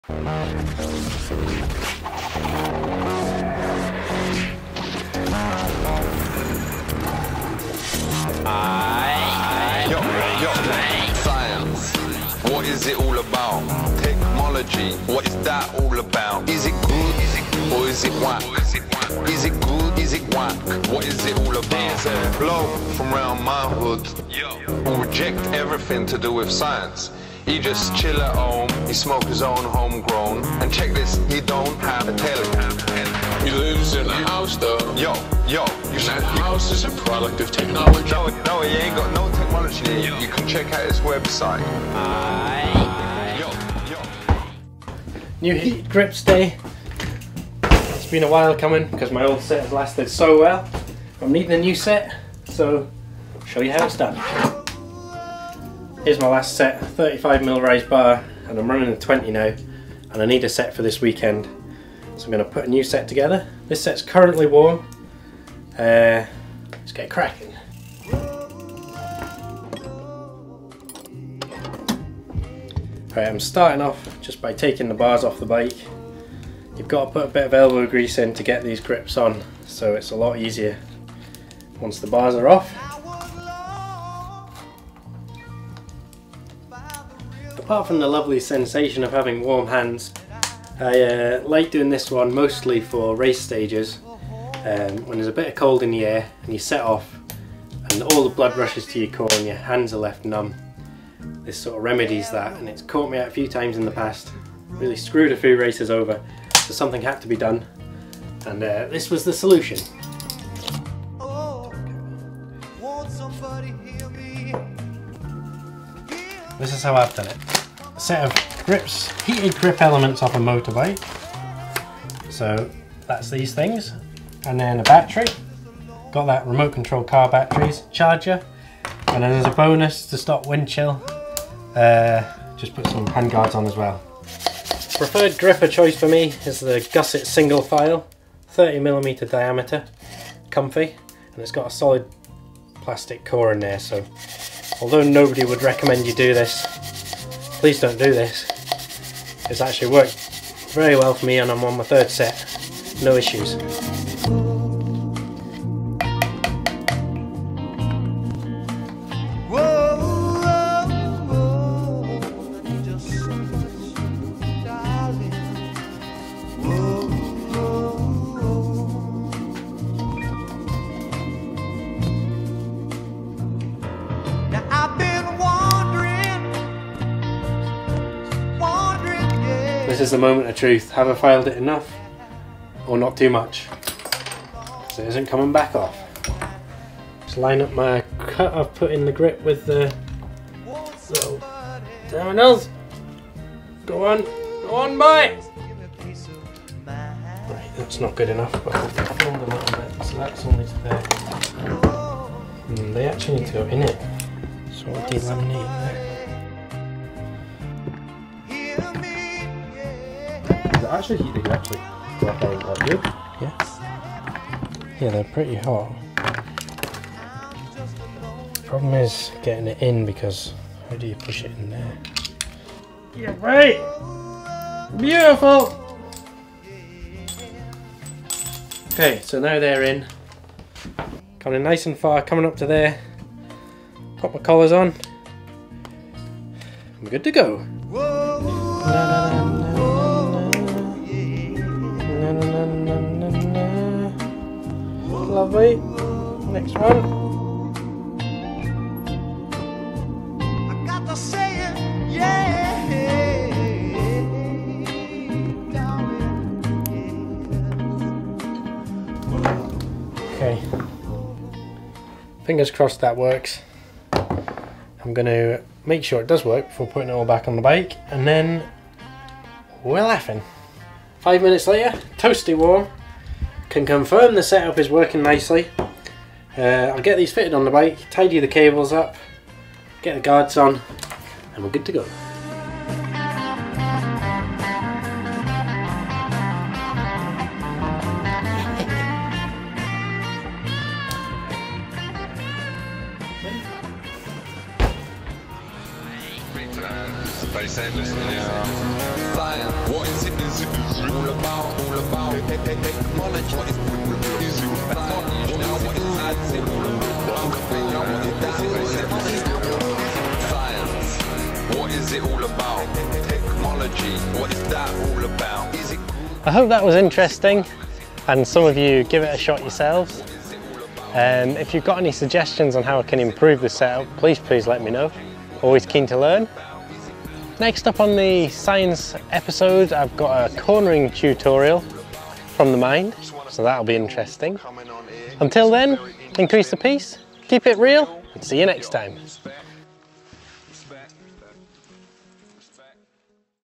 Science #3 - How to Build Heated Grips
Build a cold weather bike set up like in this video. No more cold hands. EVER.

It's an easy trick that costs around £40 for the electronics. Ideally, you'll put your heated grips on a spare set of bars so you can keep them built up and swap them out for multiple winters.

A total game changer for racing in the alps when the weather turns or just keeping the fingers alive for a cold lap round the local.